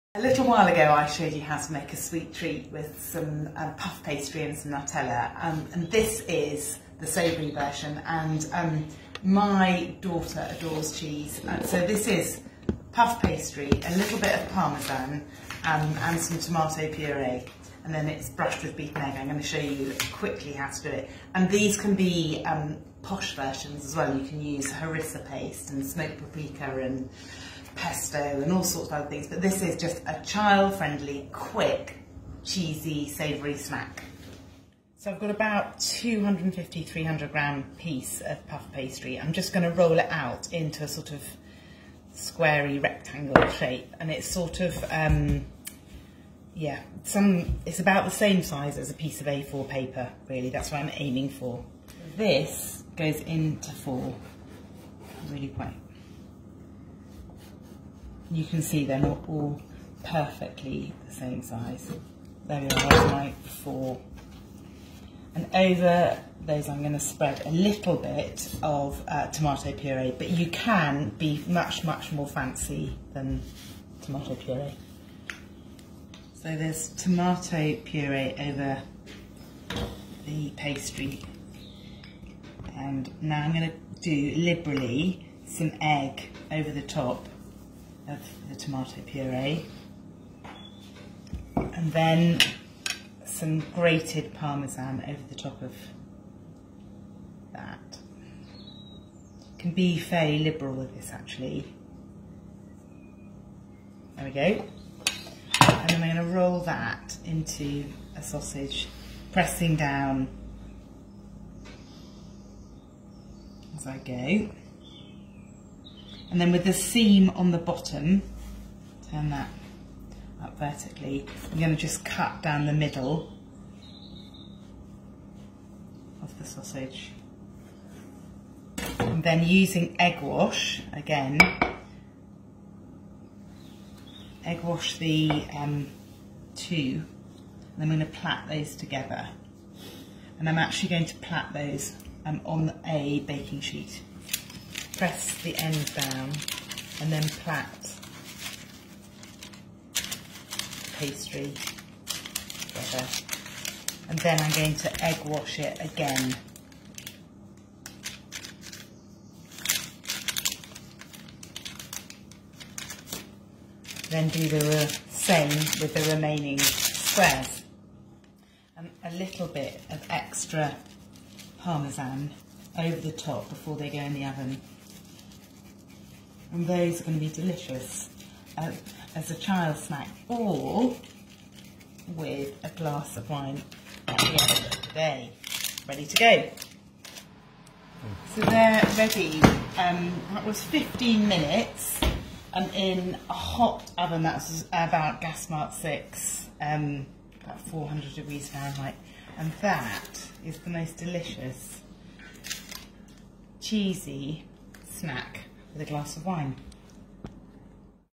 Tomato & Parmesan Twists
Looking for a little savoury treat? Camilla’s Tomato and Parmesan Twists are the perfect weekend treat to enjoy in front of the TV with a glass of your favourite fizz or in the garden as an afternoon snack.